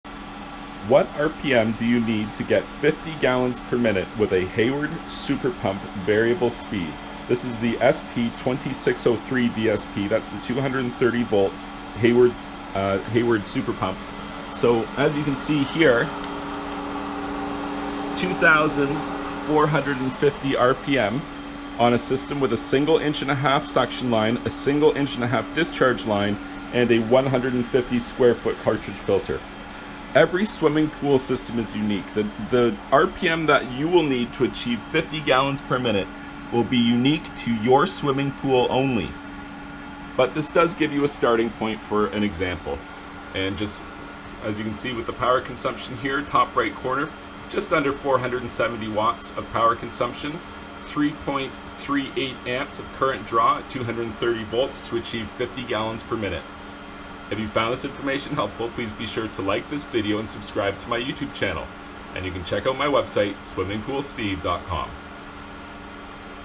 How Many RPM For 50 GPM On A Hayward Super Pump VS?
- The test in this video is an example of how many RPM is needed from the Hayward Super Pump VS to get a flow rate of 50 gallons per minute (GPM). This test system used for this video is 1.5" PVC pipes, a Hayward Super Pump VS paired with a 150 sqf Hayward cartridge filter.

We reach 50 gallons per minute at 2450 RPM and 469 watts of power consumption. When trying to find the motor speed for your system to get to 50gpm, remember that the values in this test will be different than the numbers for your swimming pool system. You can, and should, install a flow meter so you can monitor the actual flow rates through your filtration system and dial in the RPM of your variable speed pump for maximum efficiency.

The following video is a helpful breakdown of how utilizing your variable speed pool pump helps to increase efficiency and decrease cost:

These videos have helpful information about using flow meters and what they do for your pool: